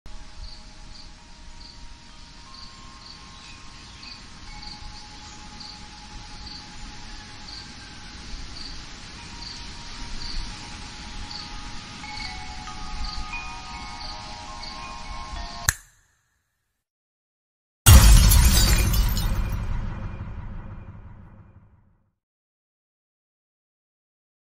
*Old* Violet Night Teaser Trailer
After months of planning, Violet Night is ready to go into full production. I’ve been thinking about making a series of my own, and I think it’s the right time to start working on making it happen.

As far as progress goes, I have two episodes fully written. I will hold auditions later down the line as I will need more help for this project.